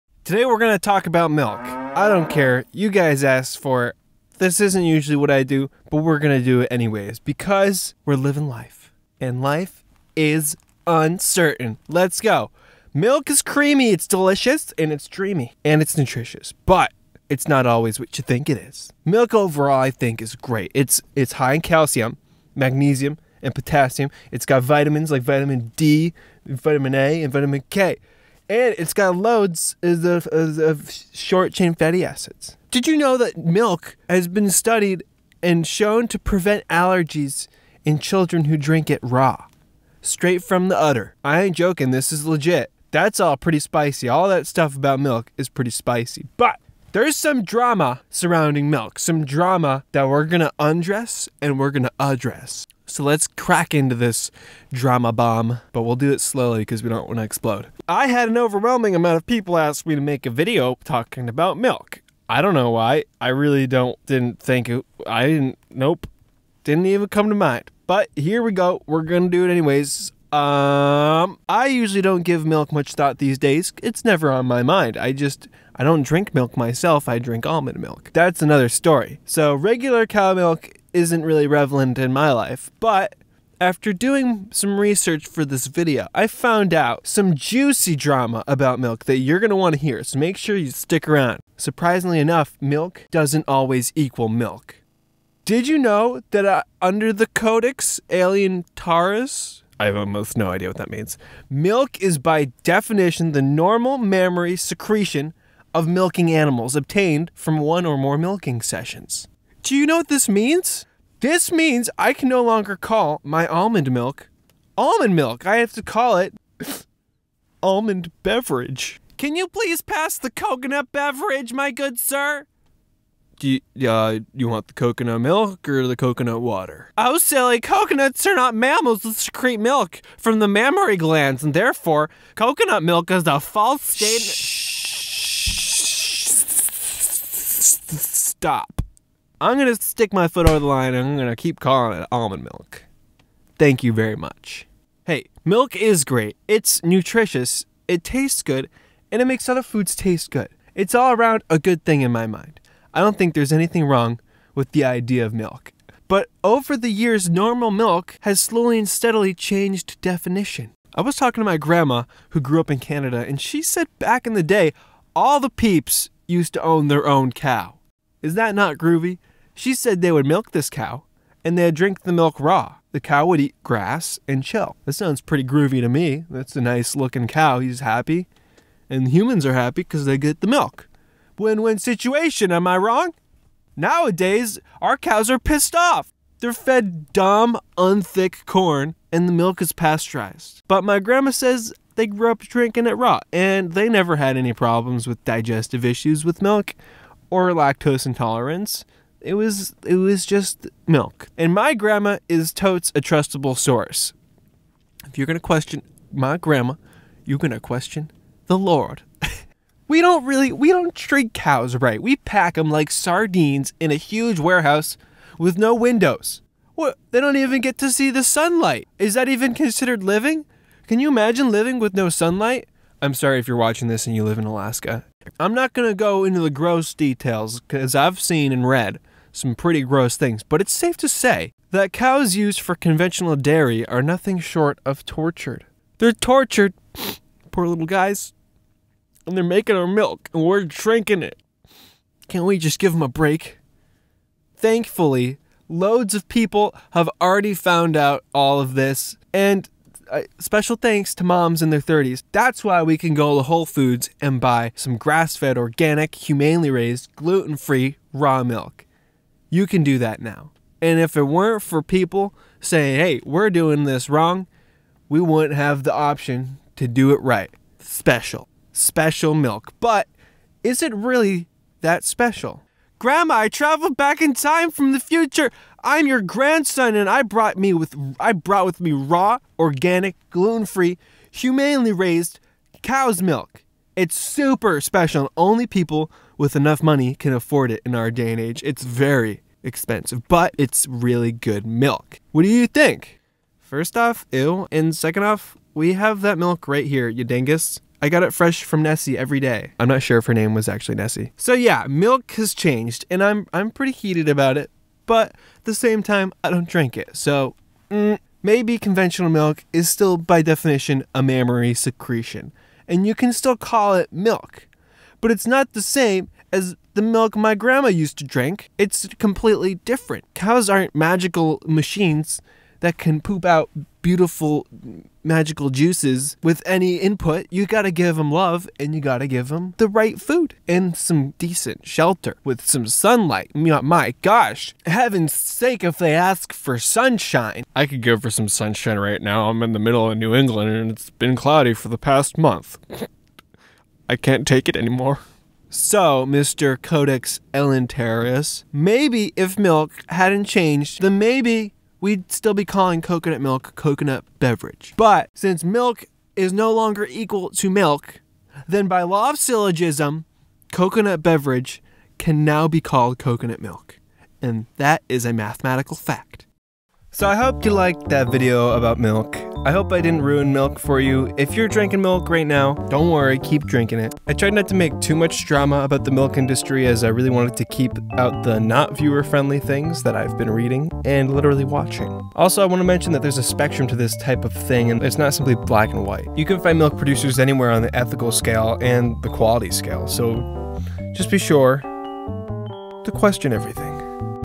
Let's talk about milk 🥛
You guys asked for it, so I made it.

Im continuing my 3 hour talk about milk 🥛, here is the first 6-7 minutes of it.

I think you're rad for reading this.

i think your extra super rad for reading this.

ok your literally awesome.

hahaha why are you still here.

i mean its only the description hahaha, but im so happy you're reading this.

I want you to put this (･ヮ･) in the comments if you made it this far.

you should really try almond milk its really tasty.

remember MALK? gosh danggit josh pour the man a glass of MALK. hahahaha (ಥヮಥ)